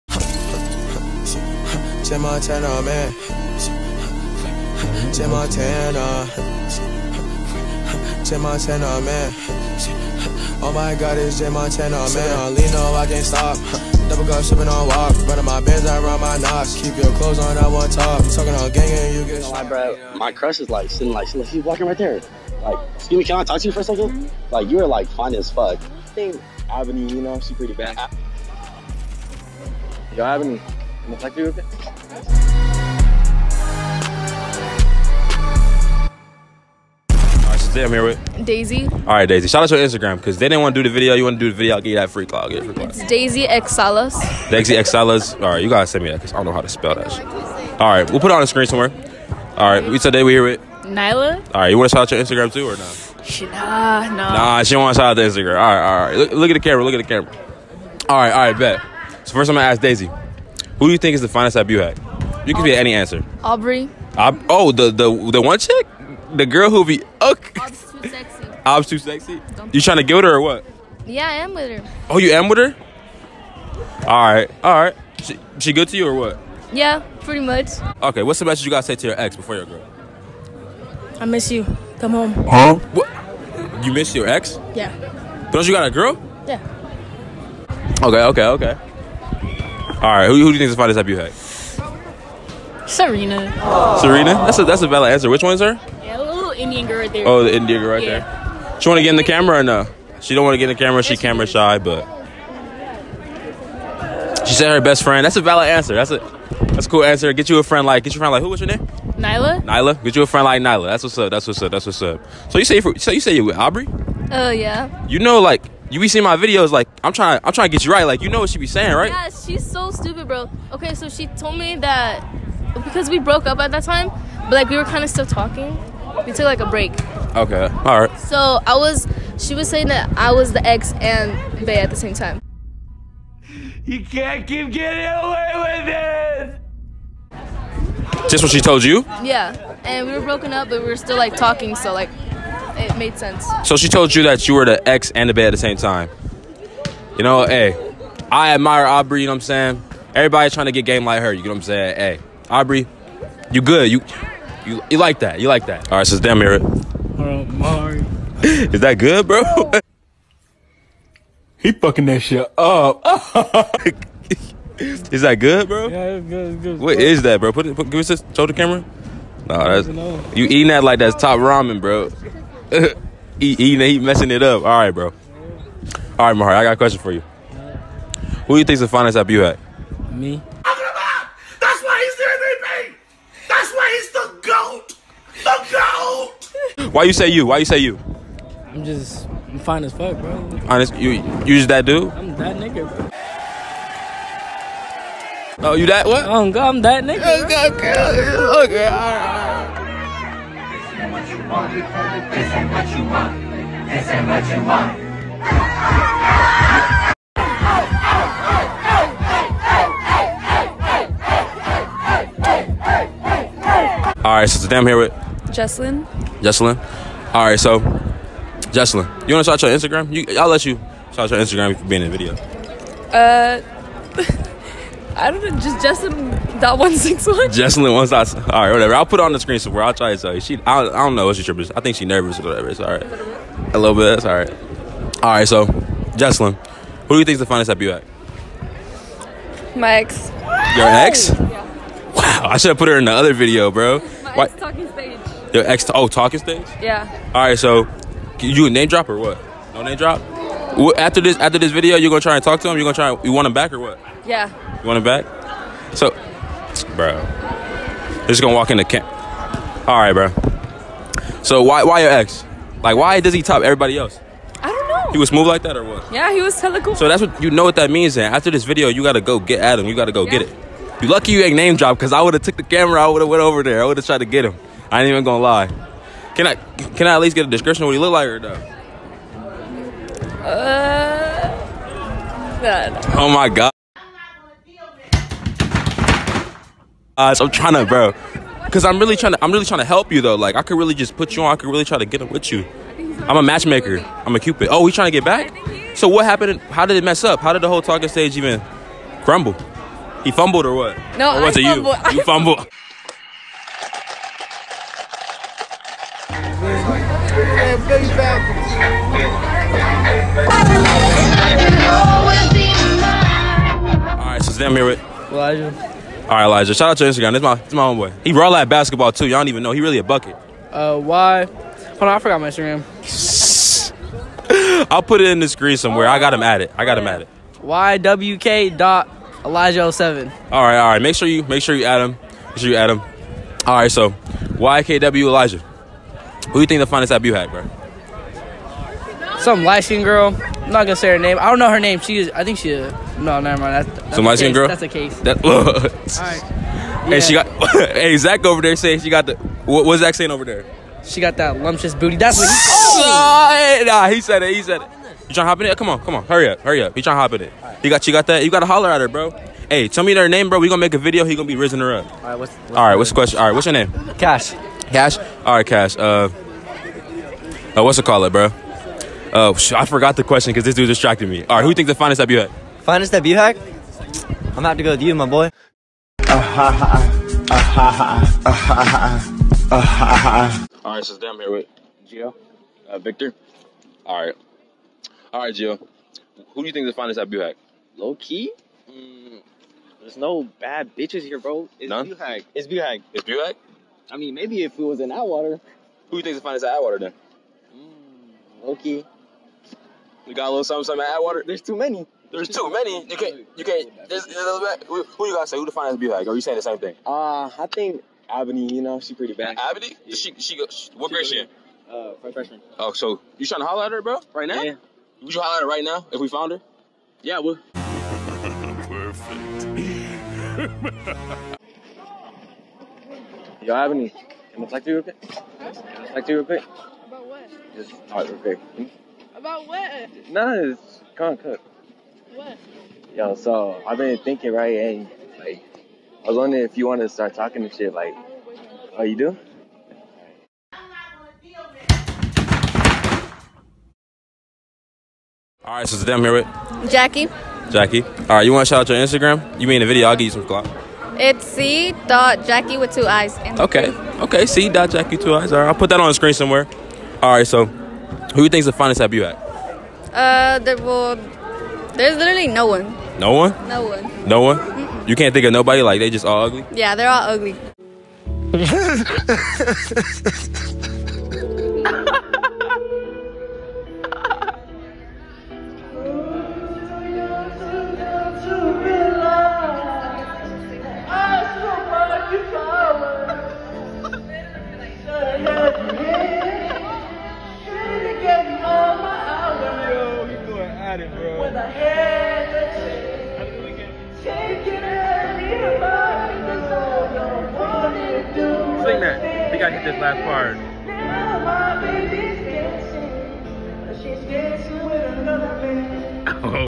Who’s The Finest At Buhach Colony High School? (He Rizzed up his Crush) Public Interviews
Public Interviews At Buhach Colony High School Central Valley Atwater California

We don’t condone any type of bullying in the video.

Who’s the finest at Buhach Colony High School
Some people even tried to get with there crushes

Rafael Social: @beaner_rafael on Instagram
Michael Social: @sr_big_michael on Instagram